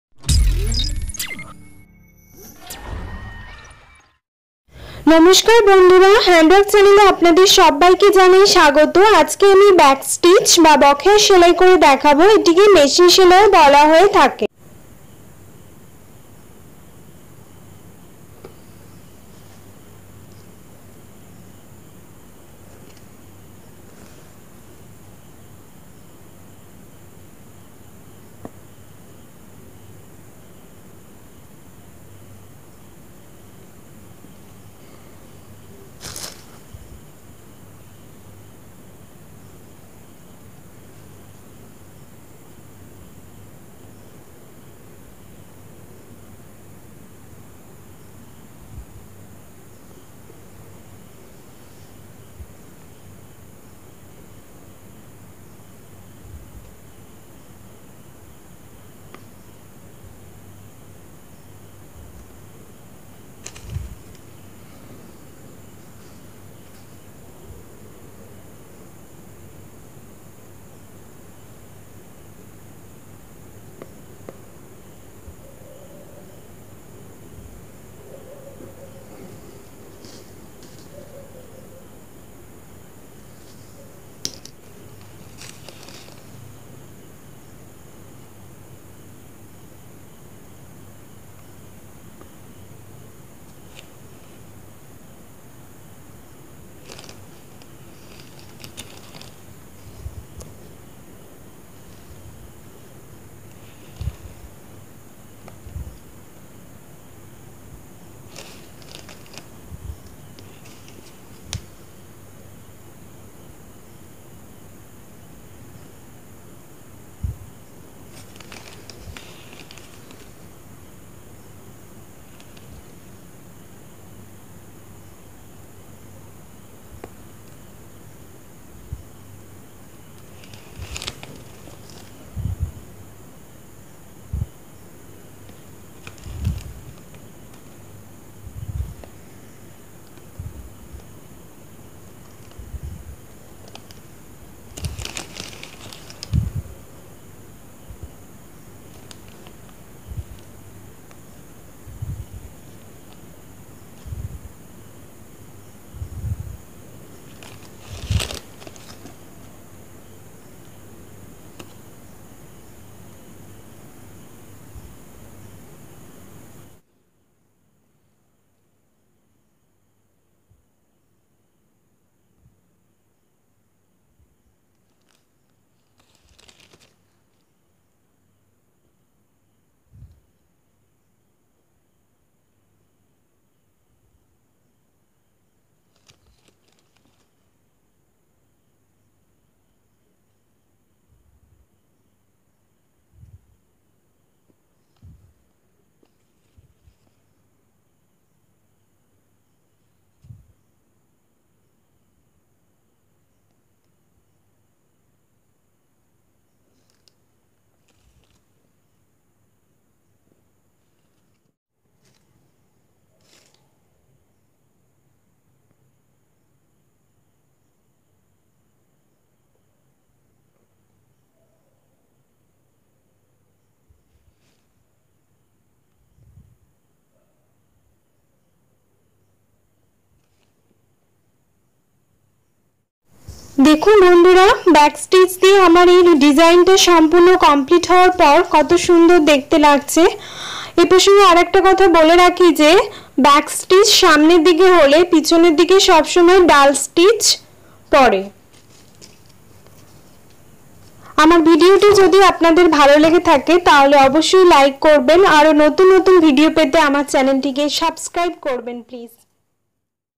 Back Stitch || Bokeya Stitch || বকেয়া সেলাই || How to stitch || Embroidery || Machine Stitch
Hi friends, welcome to my chanel.Today I gonna saw you how to sew Back Stitch or Bokeya Stitch. It's also called Machine Stitch because it is very long lasting stitch.

Bokeya#BackStitch#MachineStitch#বকেয়া সেলাই#How to Stitch